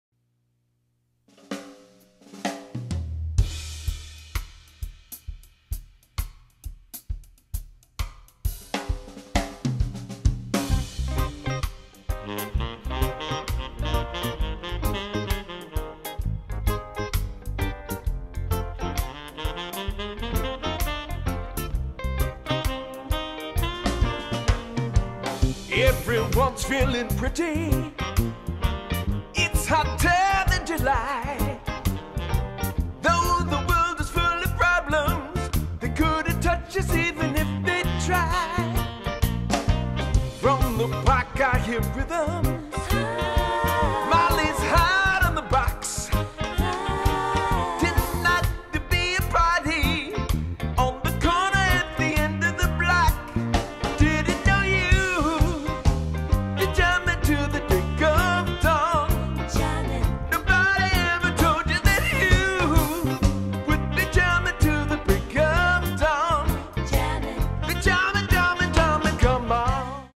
island storm band
island storm band/ boston  reggae/soul

sample of our new demo.